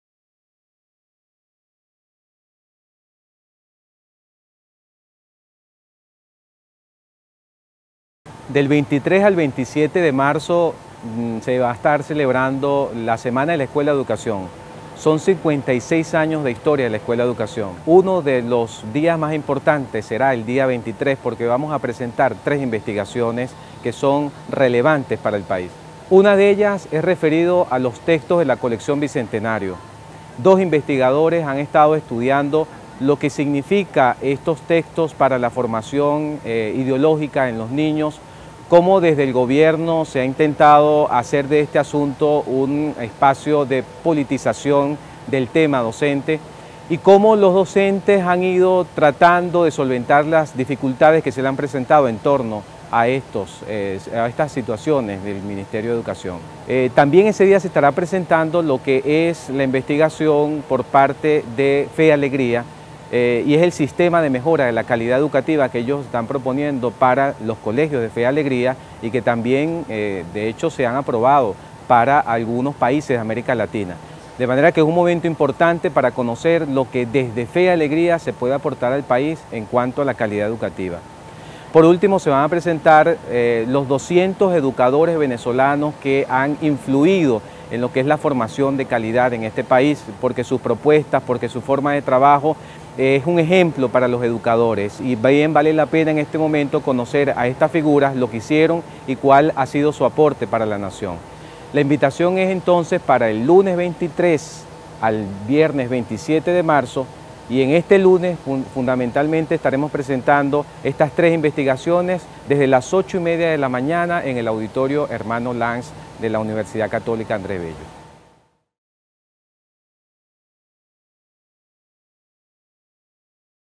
Invitación a la XXI
El director de nuestra escuela de Educación, José Francisco Juárez, invita a la comunidad a la XXI Semana Educabista del 23 al 27 de marzo. Especialmente al foro "Propuestas de Evaluación del Sistema Nacional Educativo" en el marco de PropuestasVzla2015 el lunes 23 de marzo en el Auditorio Hermano Lanz.